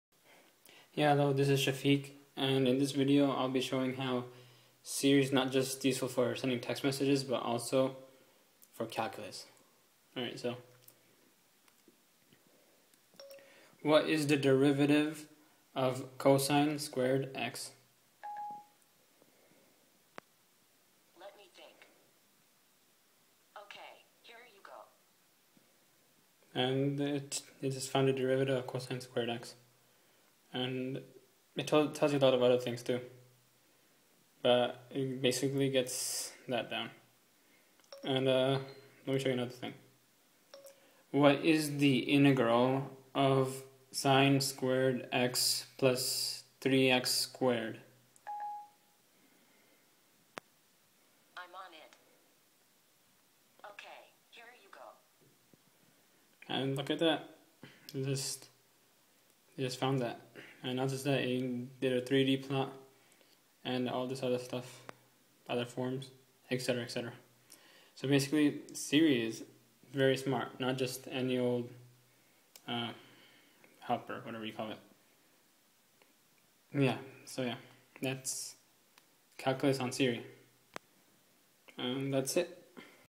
Siri Voice Assistant can do Calculus on iPhone 4S!
In this video, I show that Siri isn't just useful for voice to text messages--it can also do Calculus!!! In this video, I took a derivative and an integral of two trigonometric functions to also show that it can do much more than simple calculus!